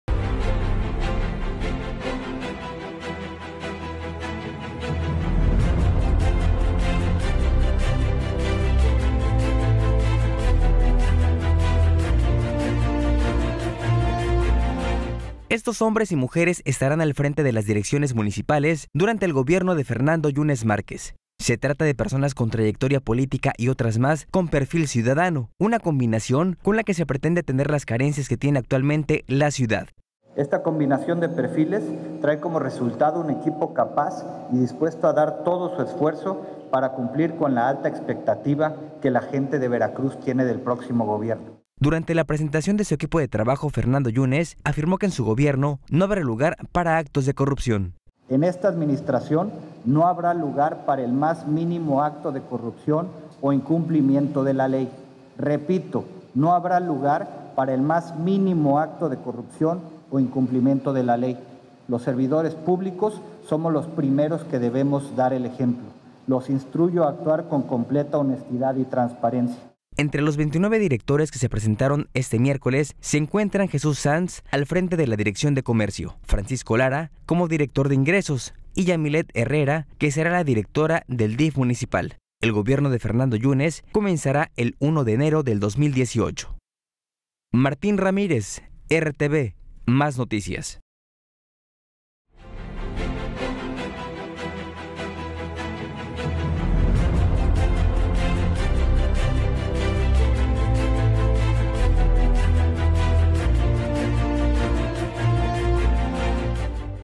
Alcalde electo de Veracruz presenta parte de su equipo de trabajo
Este miércoles ,el alcalde electo del municipio de Veracruz, Fernando Yunes Márquez, presentó al equipo de directores que lo acompañará durante su gestión.

Ahí enfatizó que se trata de personas jóvenes y comprometidas con mejorar la realidad del municipio.